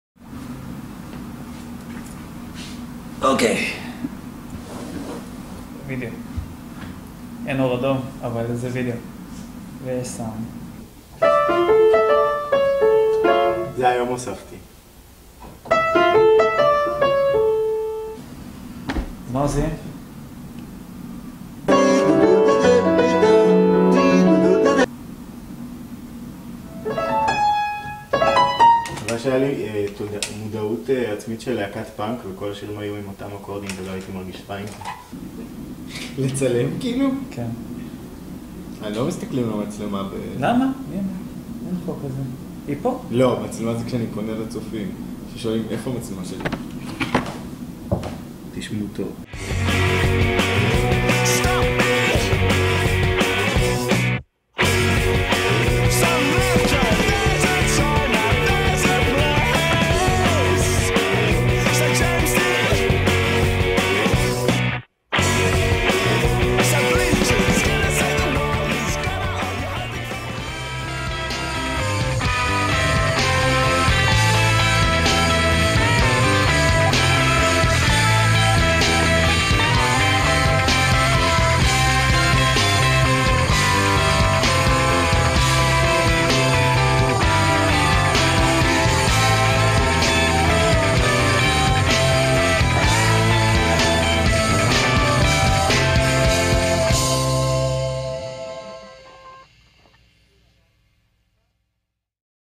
יום בחיי יובל ברוסילובסקי
הנכס הלאומי יובל ברוסילובסקי נותן לנו הצצה בלעדית לנבכי נפשו ולעולמו המרתק.
היווכחו כיצד הוא לוכד ברשתו את עלמת החן ואשת החיל יערה פילו אשר לא מעוניינת בו כלל וכלל.
הישארו עימנו.